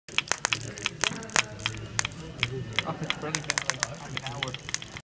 Checking Spark Gap
Increasing gap to see how well the spark plug works